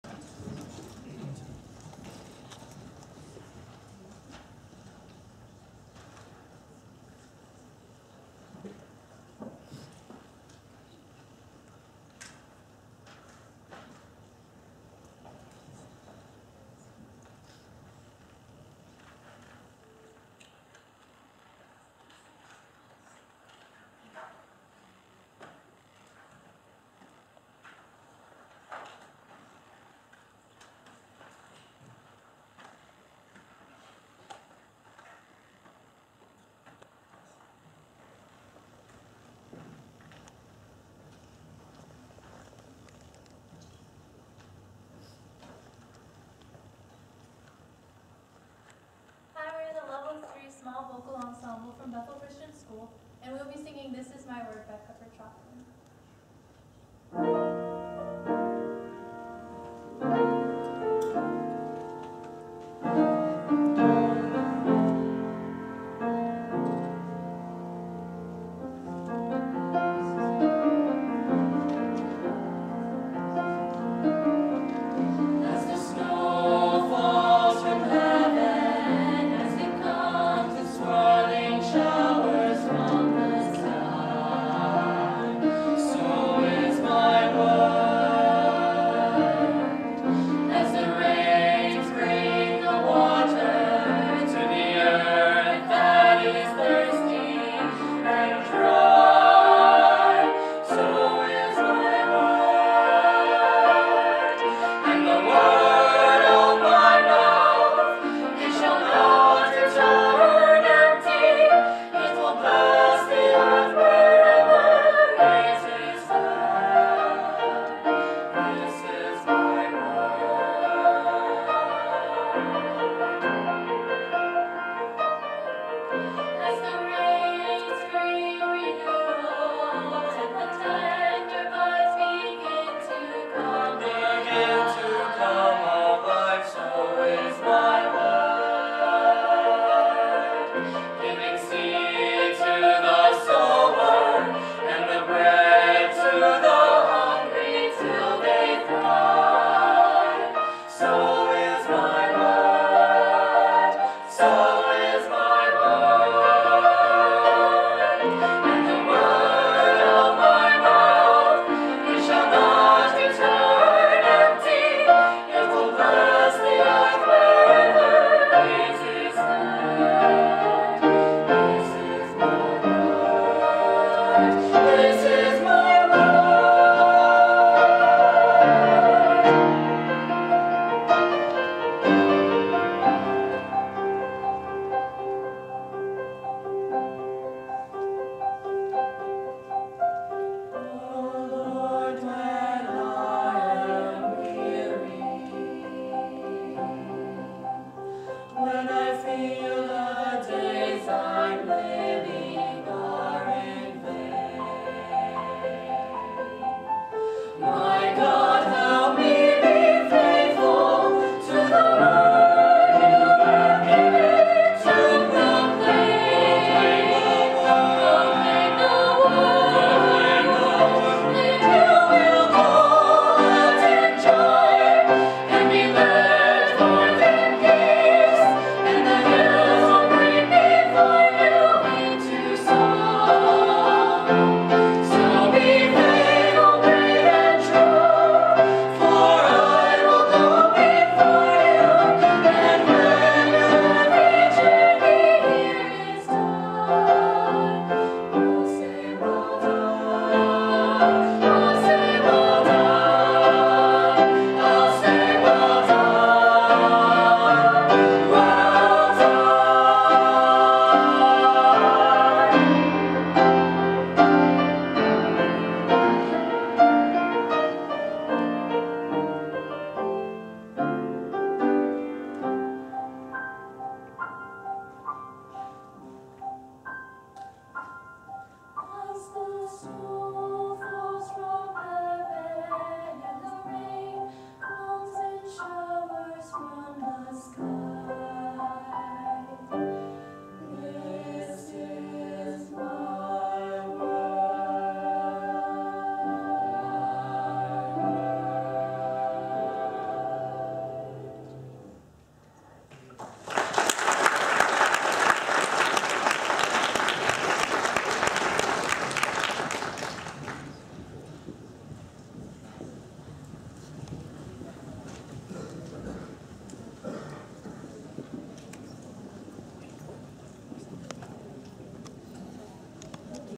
Bethel Christian School Small Ensemble
BCS Small ensemble sings at ODACS State Competition 18Mar16